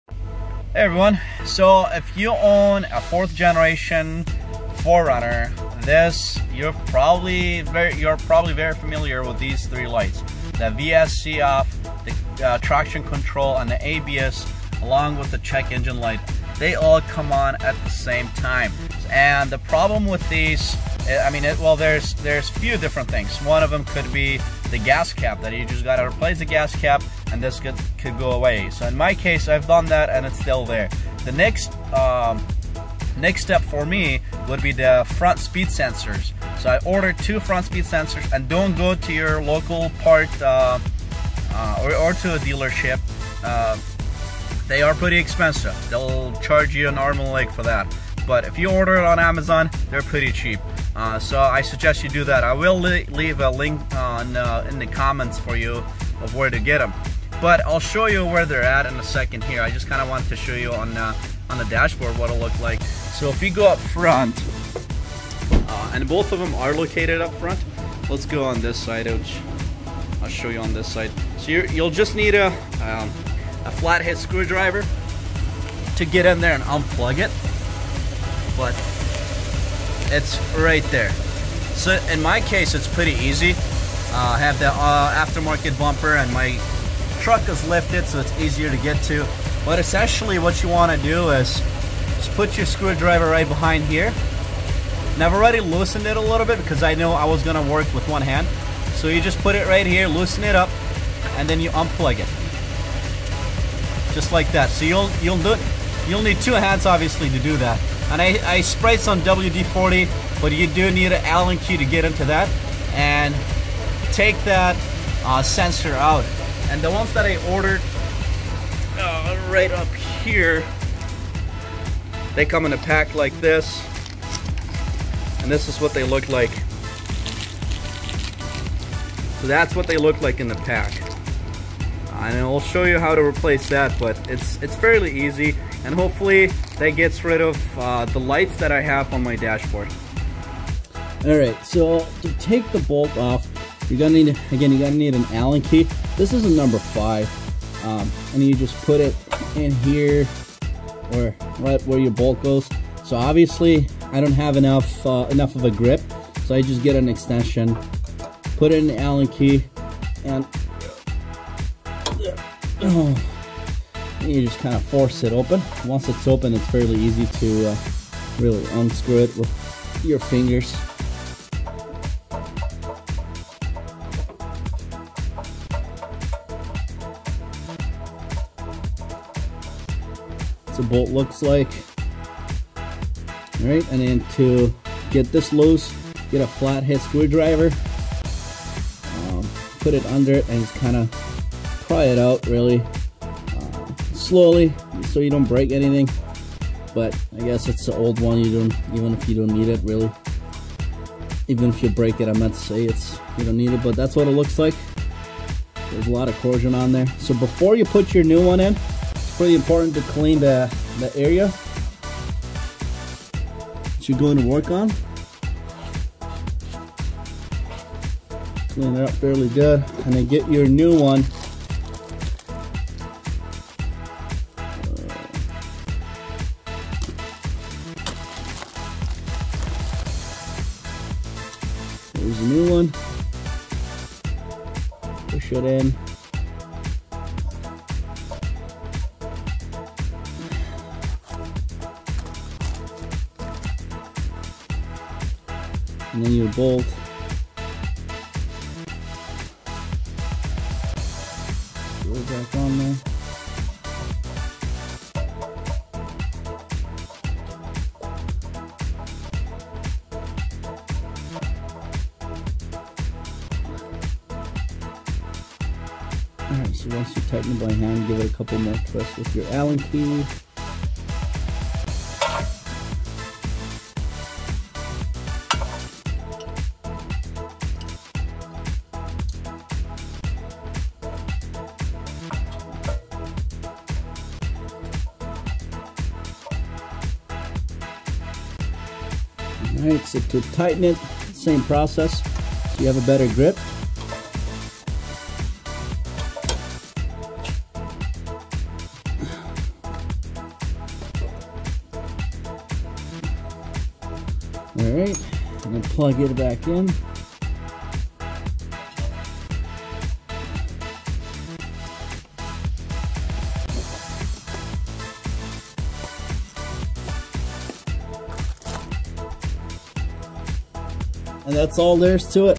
4th generation 4Runners VSC ABS traction control lights. Replacing the speed sensors
This process is for 2003 to 2009 Toyota 4runners.

Link to sensors